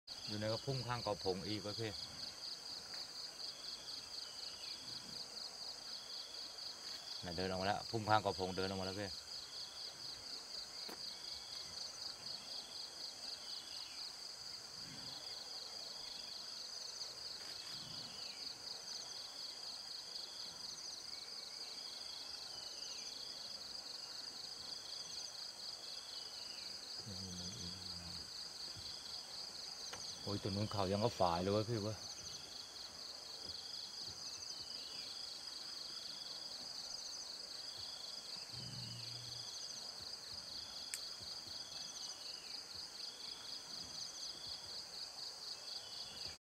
Gaur (Bos gaurus), Thailand. 20180804_061538.m2ts
Gaur (Bos gaurus) bull on the edge of forest in Kui Buri National Park, Thailand. IUCN=Vulnerable.